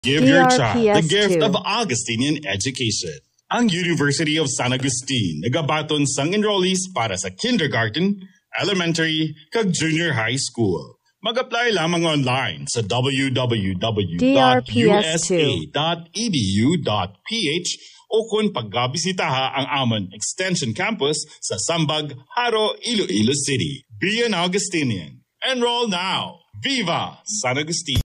University of San Agustine SY 2023-2024 Enrollment Radio Ad Q2 2023 28s (Iloilo/Version 2)
Ito po ang Q2 2023 Provincial Radio Ad ng University of San Agustine sa Iloilo - offering for new enrollees this SY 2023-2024. (Version 2)

DRPS Two 4654: University of San Agustine SY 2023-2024 Enrollment Radio Ad Q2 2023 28s (Iloilo/Version 2)